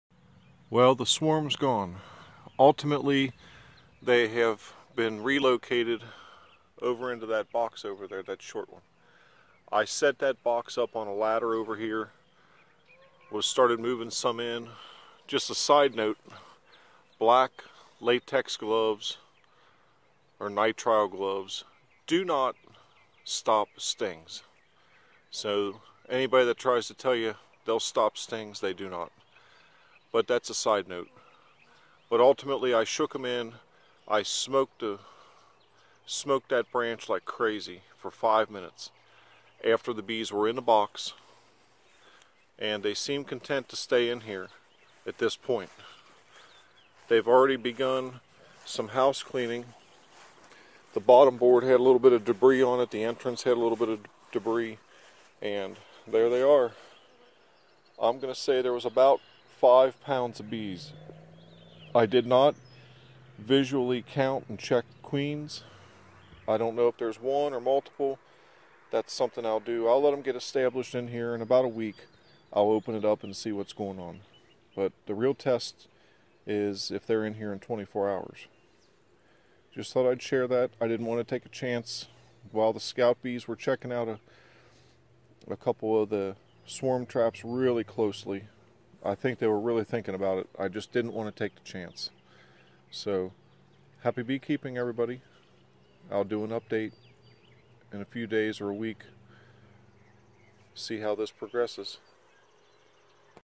Bee swarm in a new home
Rather than take a chance on the bees leaving I went ahead and shook them into a box with drawn comb. They seem to be taking up and doing some house cleaning at the entrance already. The real test will be if they are in there tomorrow afternoon.